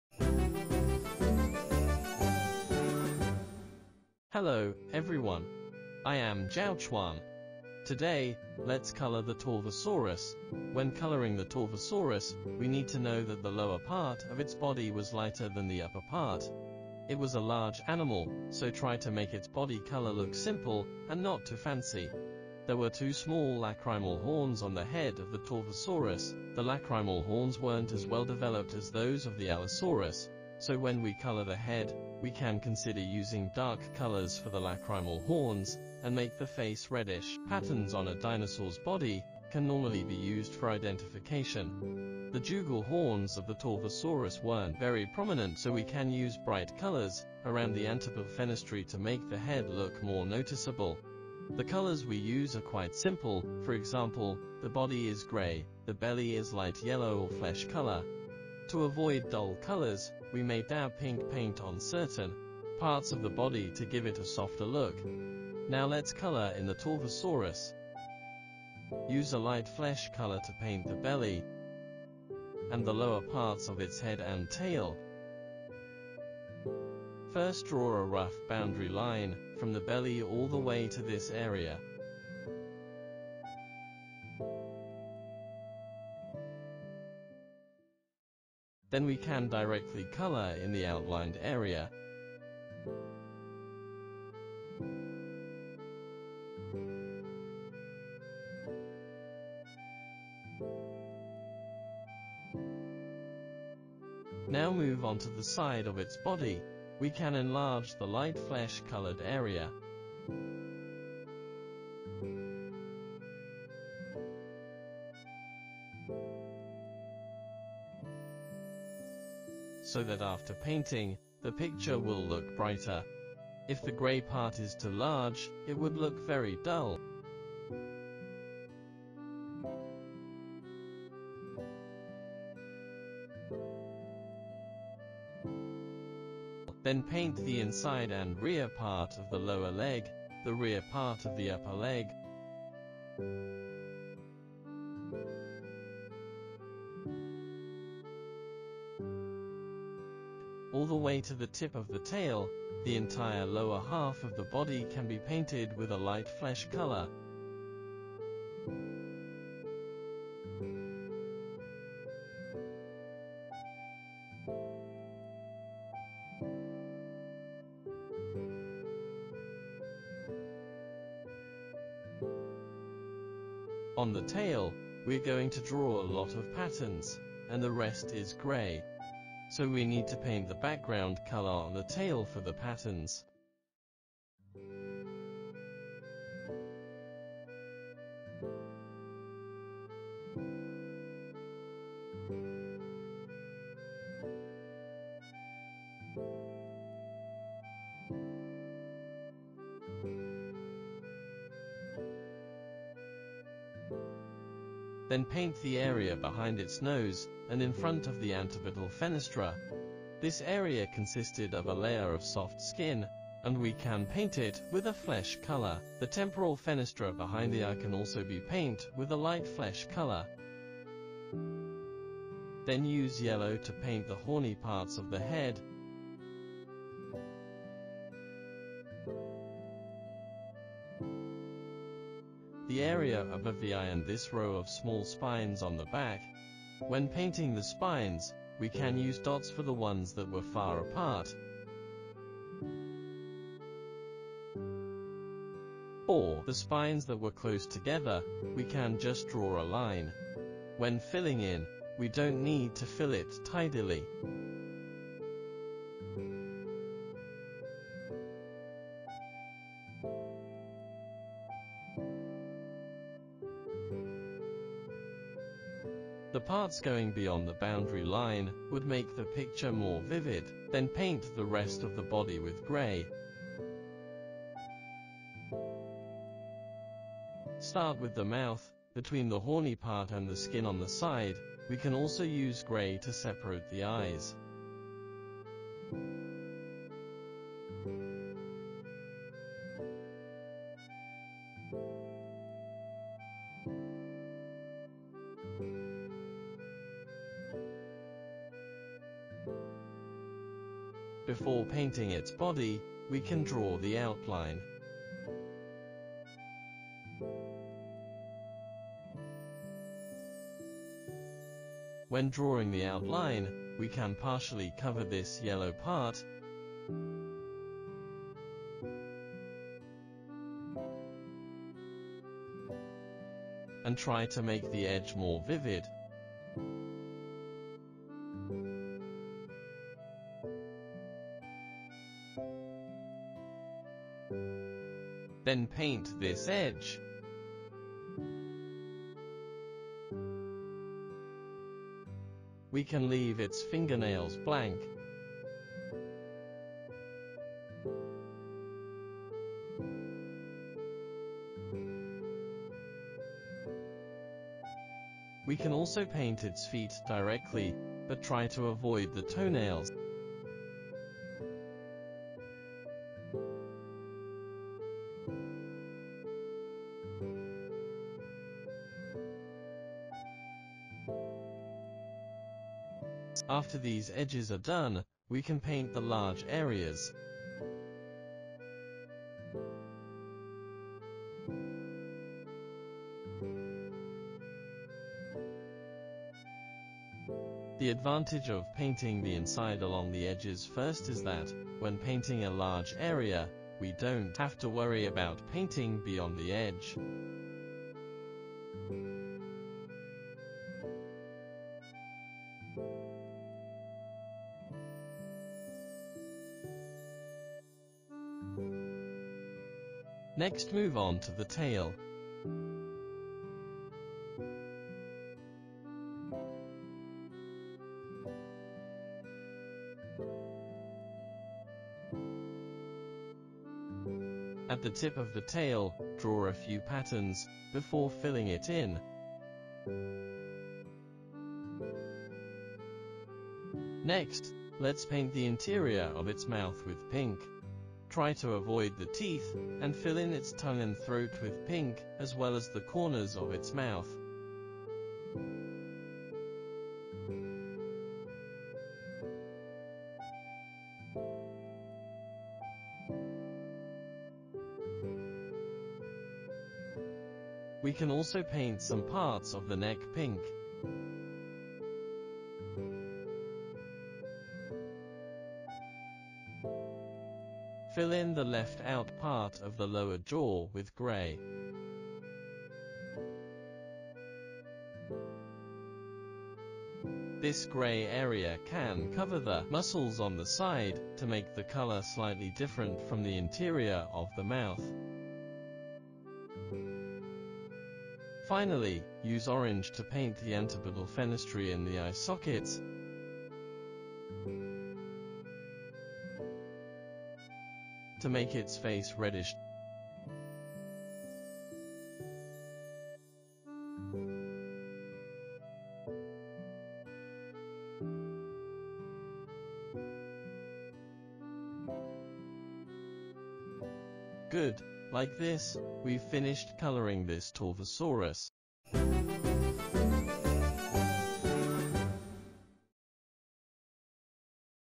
Conner the Torvosaurus 8: The Color | Learn to Draw Dinosaurs with ZHAO Chuang
The Torvosaurus was a large animal, so we should use simple colors. We can fill its body with gray and the belly with flesh color. In order to make the colors less monotonous, we can paint some parts of the body pink and use brighter colors for the hard keratinous parts. Check the video to find more.
The scientific art reconstructed model, Conner the Torvosaurus from the PNSO Prehistoric Animal Models series is available from PNSO online stores.